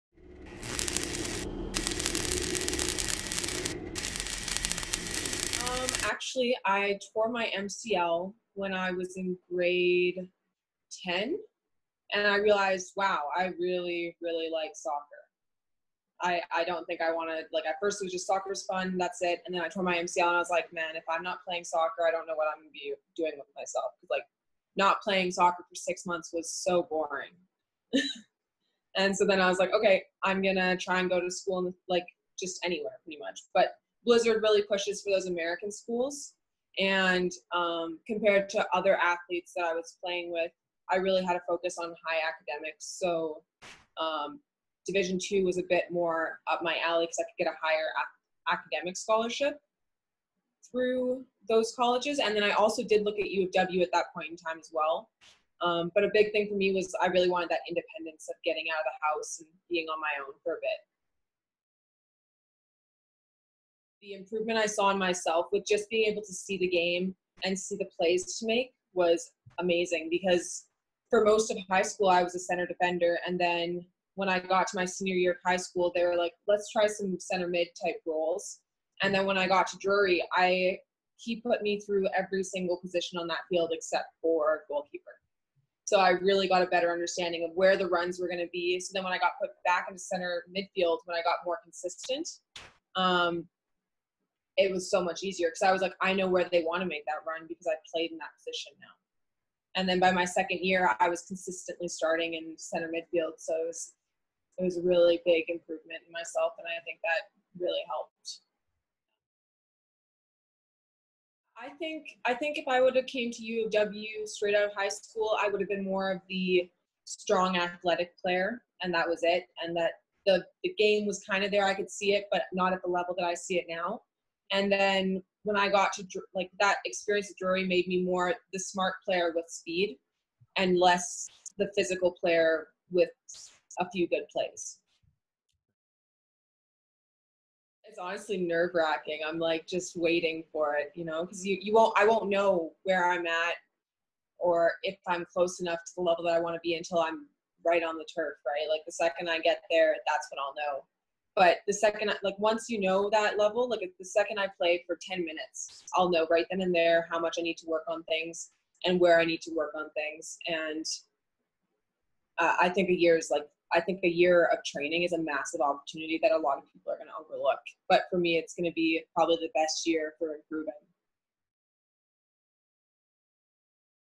Wesmen Recruiting: Karrisa Schneider
Newest Wesmen soccer recruit Karrisa Schneider, an Oak Park High School grad and NCAA D2 transfer, talks about her journey to UWinnipeg.